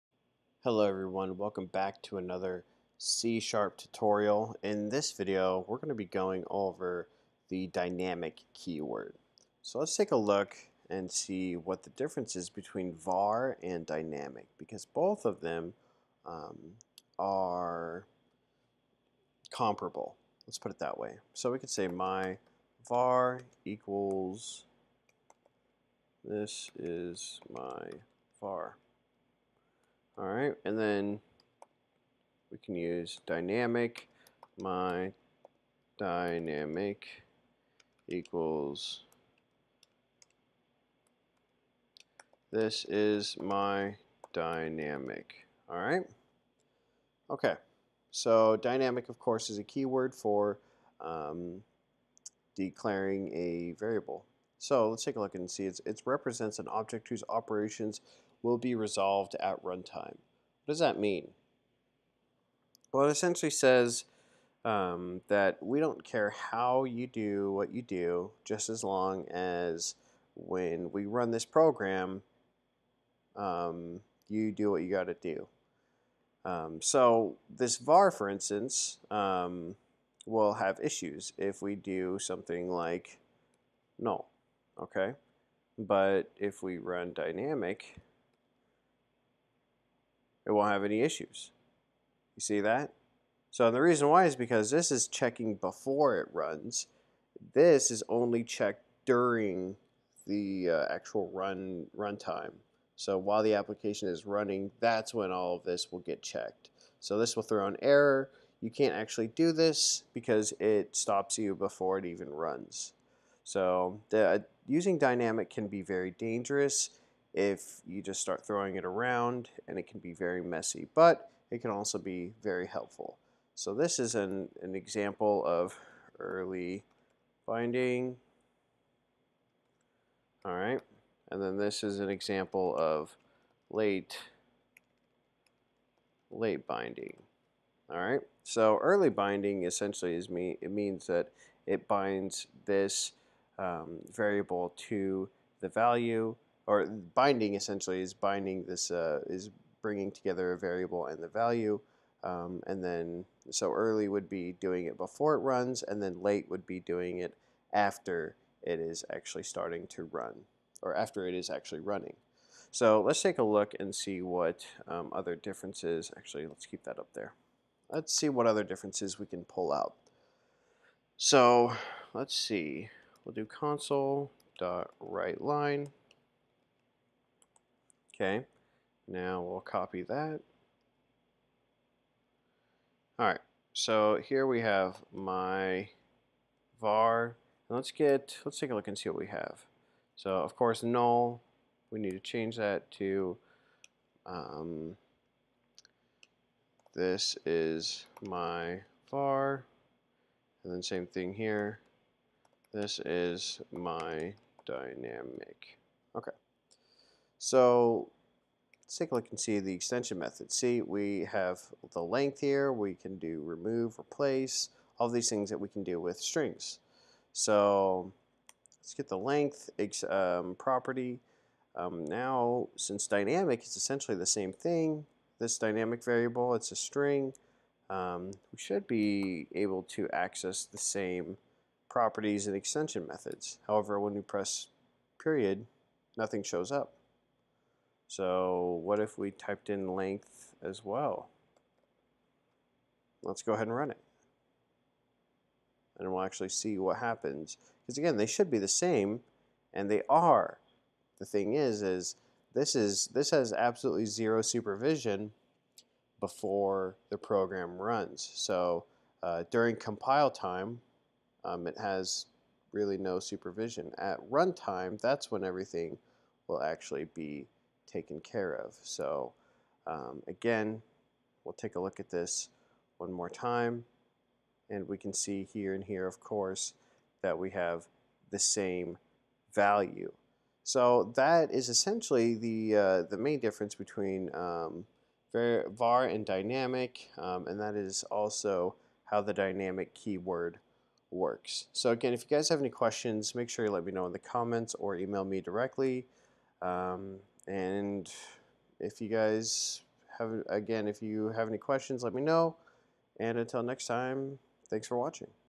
C# Tutorial: Var vs Dynamic
In this lesson I go over the difference between var and dynamic.